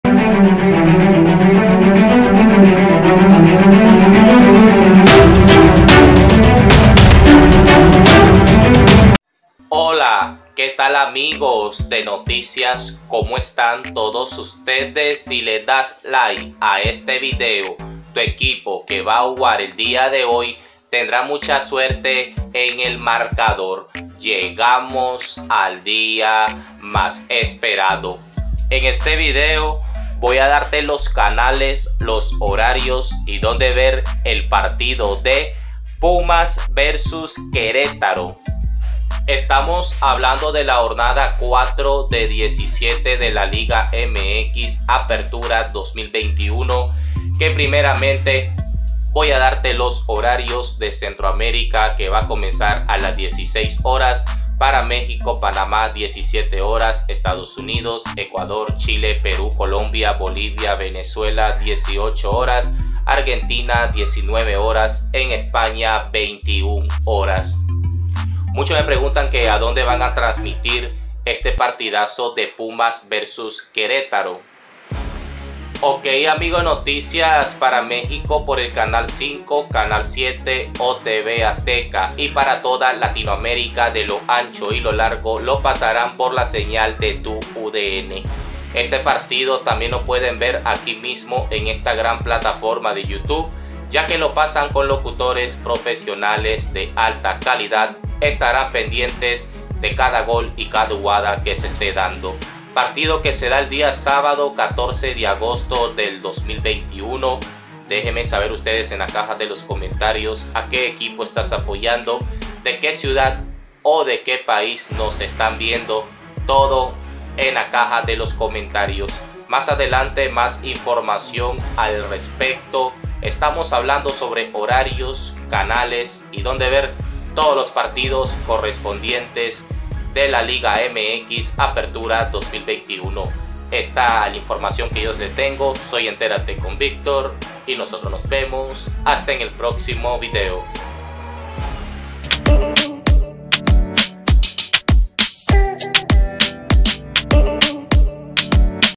Donde ver Pumas vs. Querétaro en vivo, por la Jornada 4, Liga MX 2021
Ver Pumas vs. Querétaro, Donde ver, en que canal ver, hora, día, en vivo, Querétaro vs. Pumas Sábado 14 de Agosto 2021, les dejo los canales con país y donde ver hoy el partido Pumas vs. Gallos Jornada 4 de 17 Liga MX, Apertura 2021, desde el estadio Olímpico Universitario.